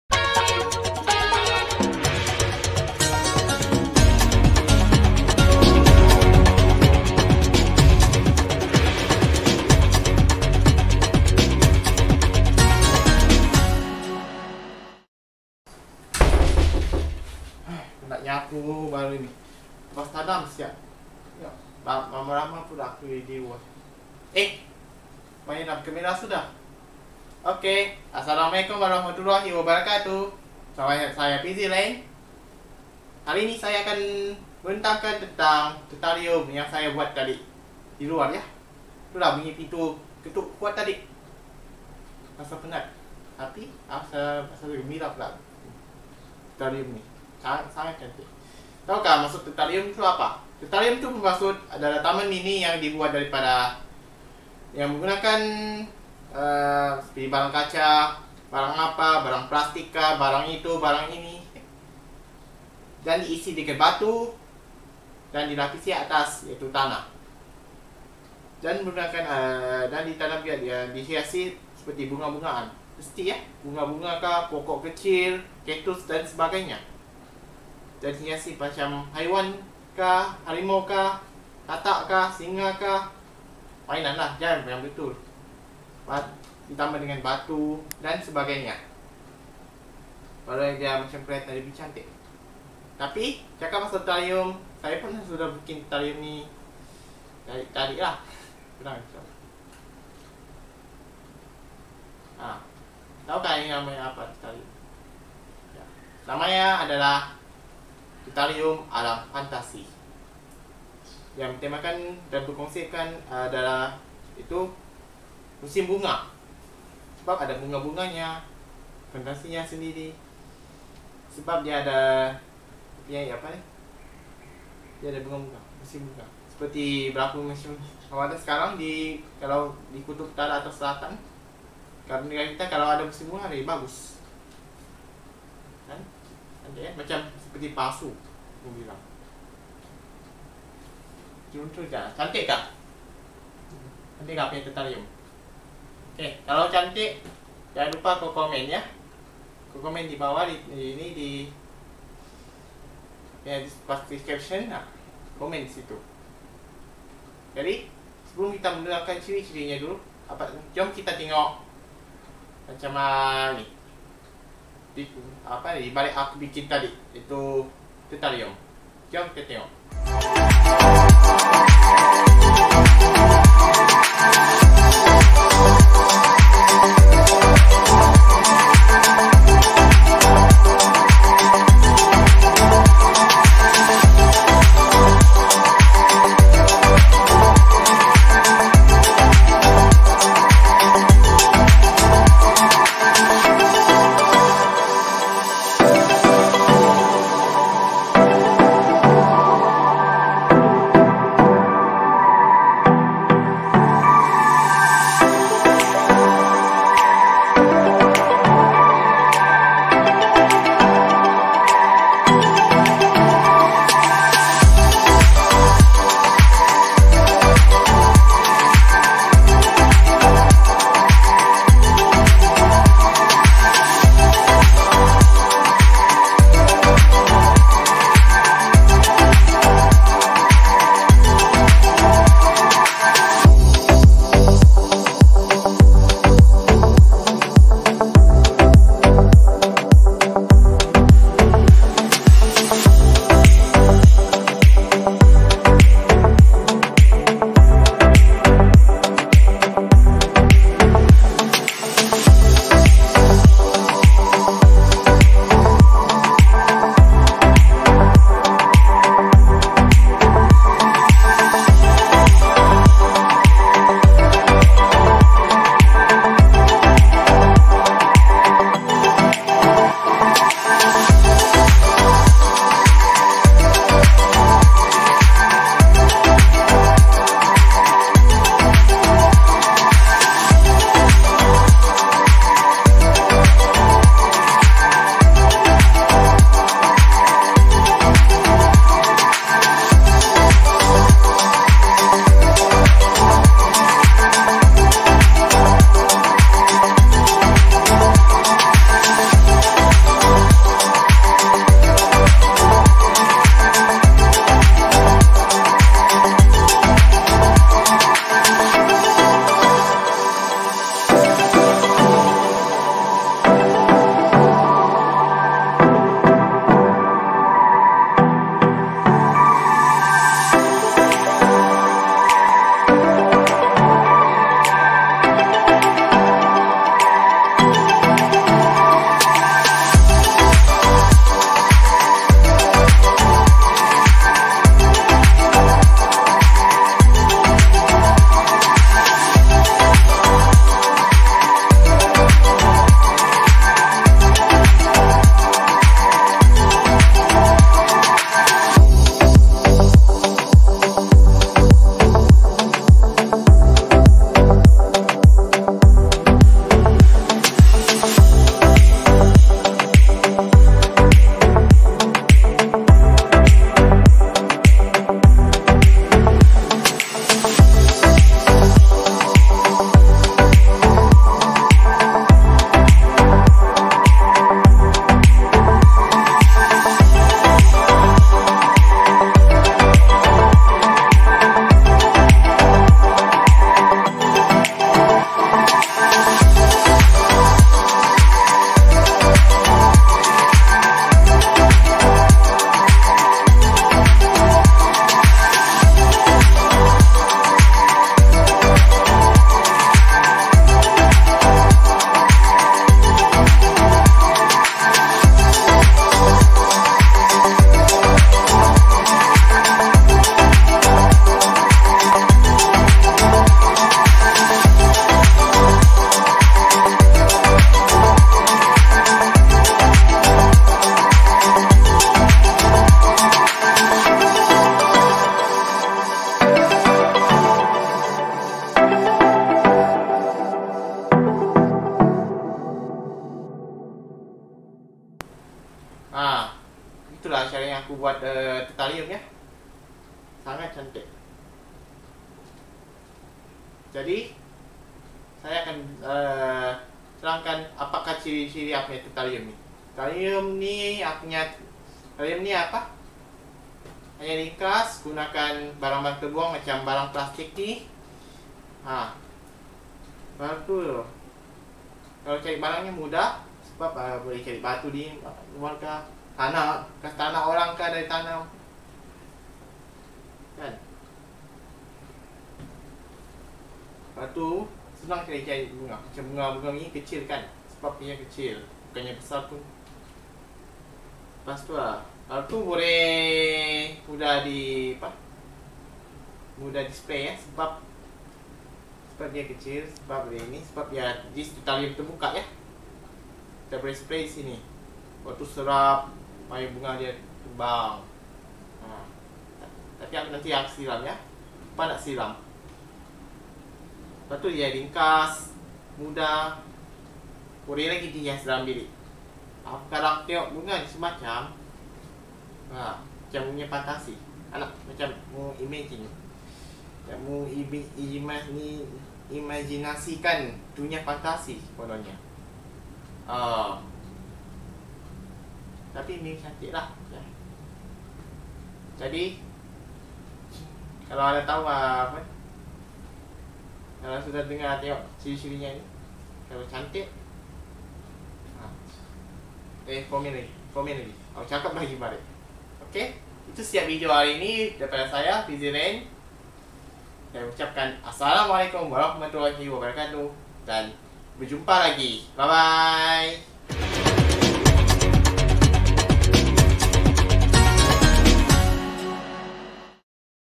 Terarium Alam Fantasi | Fizi Ren
Video ini merupakan satu projek terarium untuk mata pelajaran Bahasa Melayu. Hari ini, saya akan kongsi kepada penonton semua tentang projek terarium dan cara-cara membuat terarium.

Adakah terarium ini cantik? Tinggalkan ruangan komen anda di bawah.